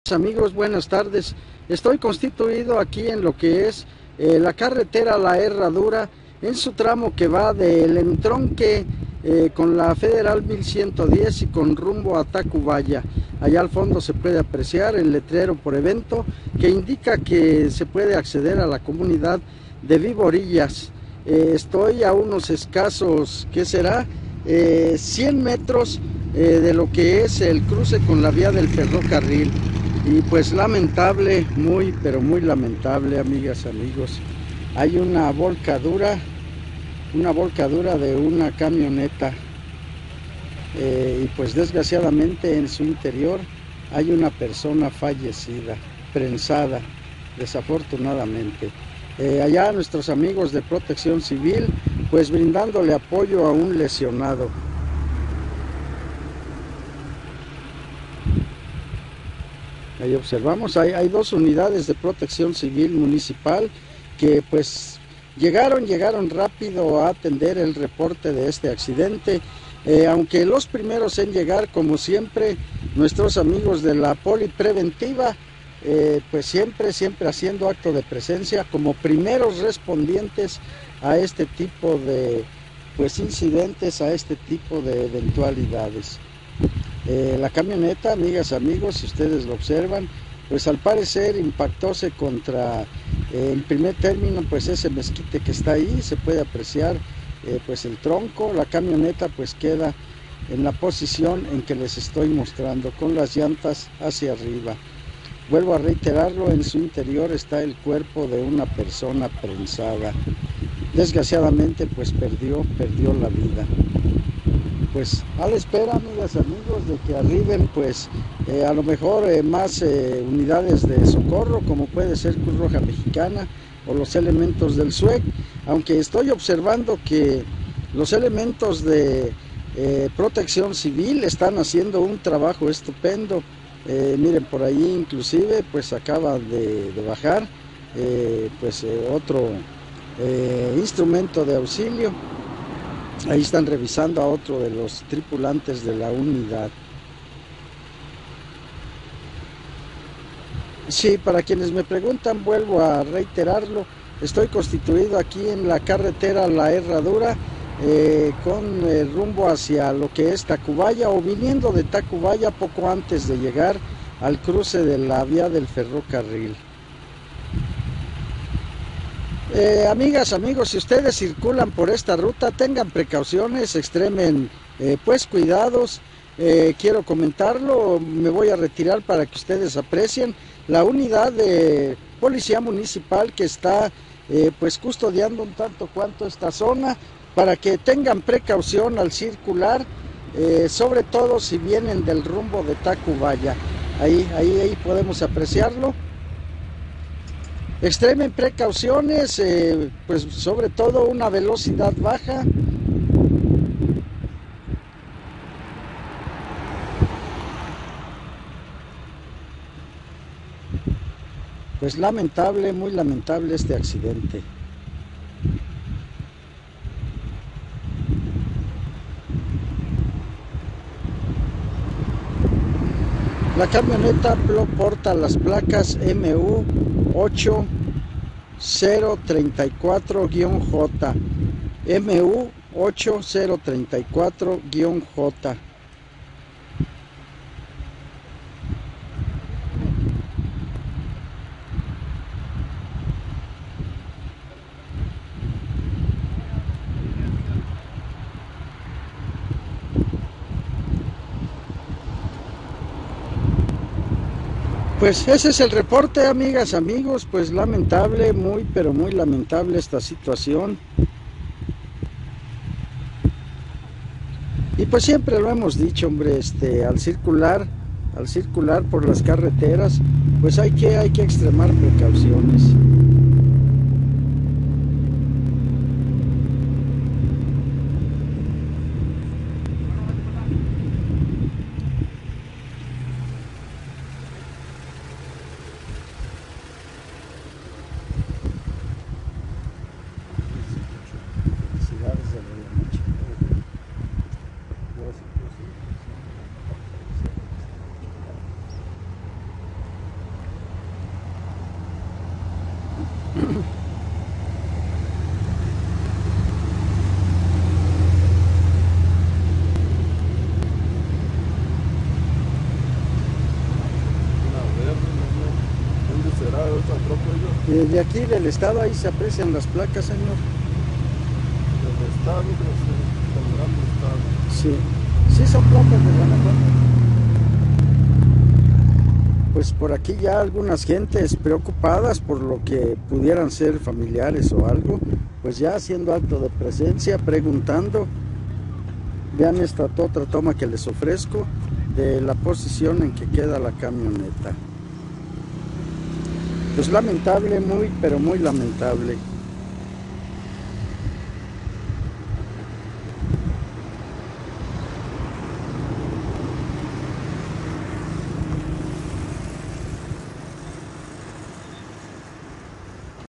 Volcadura arroja una persona muerta y 2 lesionados en Pénjamo
Carretera La Herradura, Tacubaya-Federal 1110